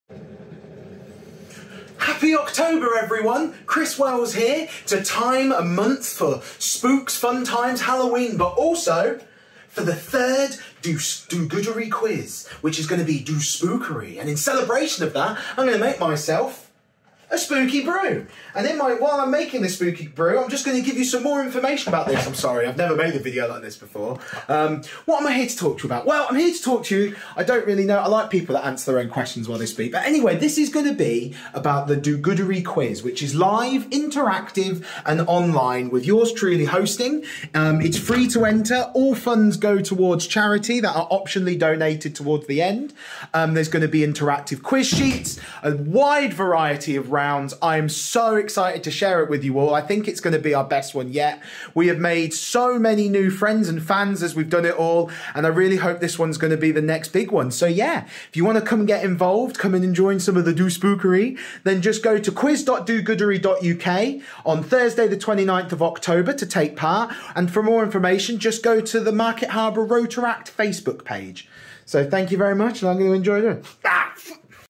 DOSPOOKERY - HE
Just a quick totally normal talky head video about the upcoming DOSPOOKERY QUIZ on Thursday 29th October!